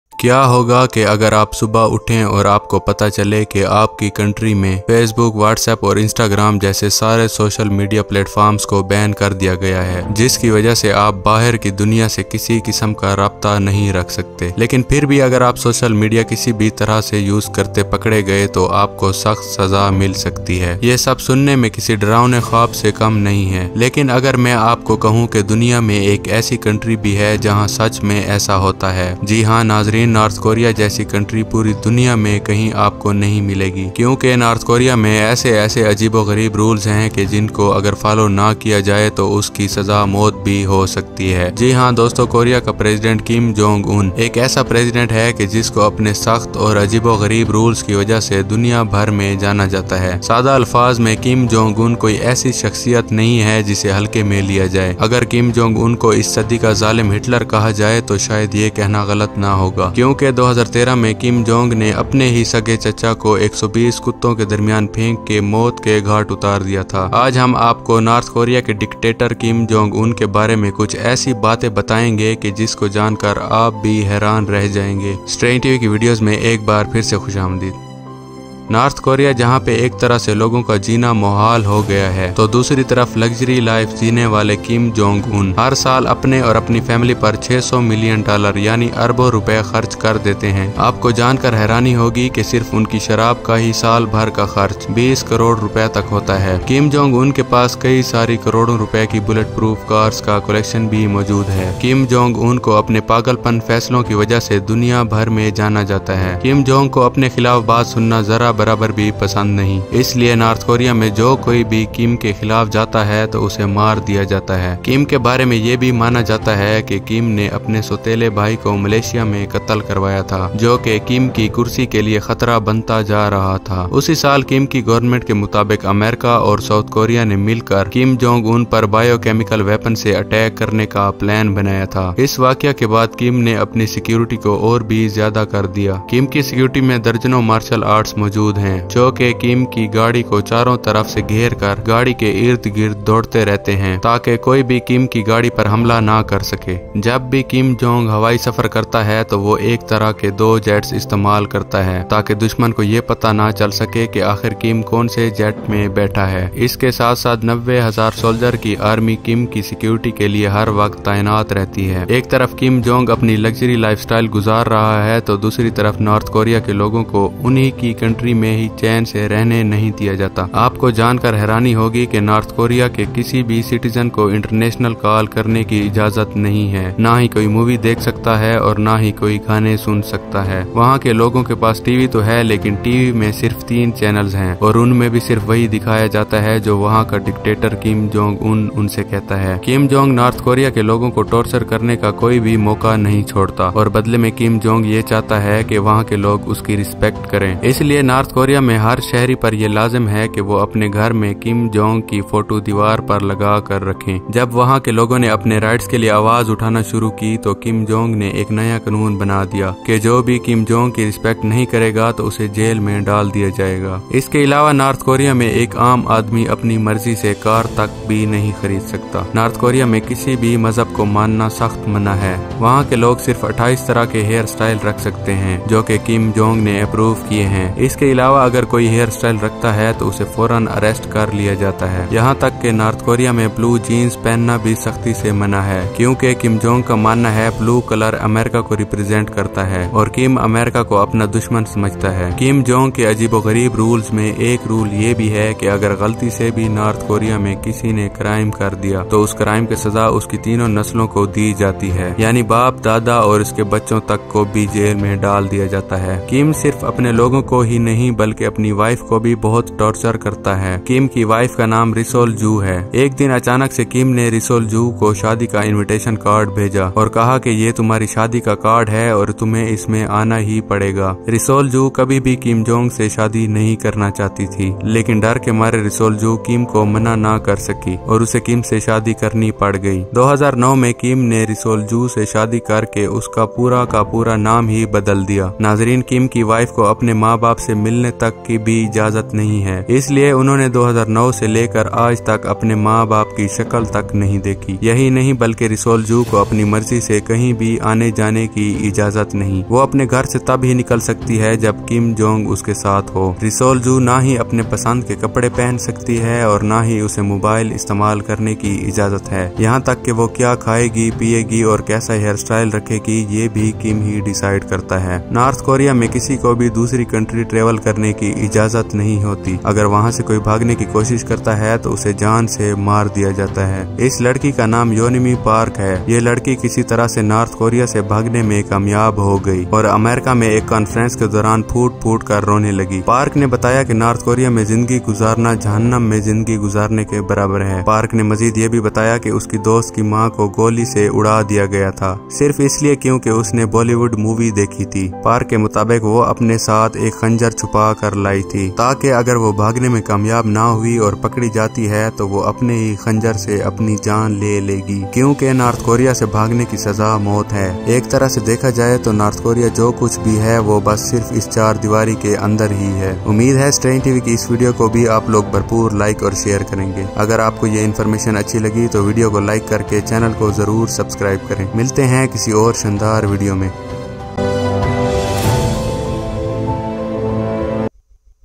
Kim Jong Un Wife Secretive Lifestyle | Strange Rules In North Korea
North korean dictator Kim Jong Un is famous for his strange rules in north korea .In this video we will se how life is difficult in north korea and how Kim jong un also the president of North korea 🇰🇵 living his life under the strict security of his force . Hopefully you will like the video .